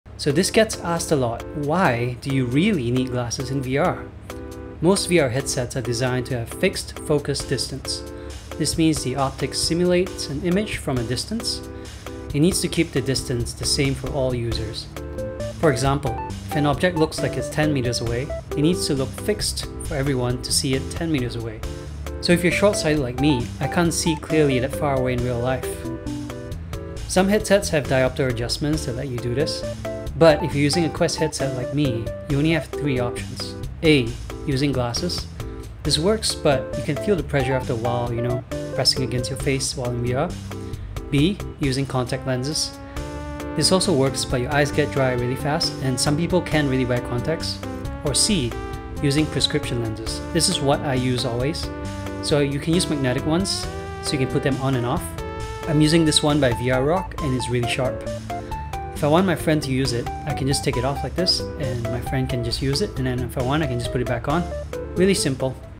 Why do you REALLY need glasses in VR?
A very short explanation on why you need glasses in VR!
*I forgot option D) Laser surgery 🤪

If you're looking for a discount for prescription lenses, you can use this one by VR Rock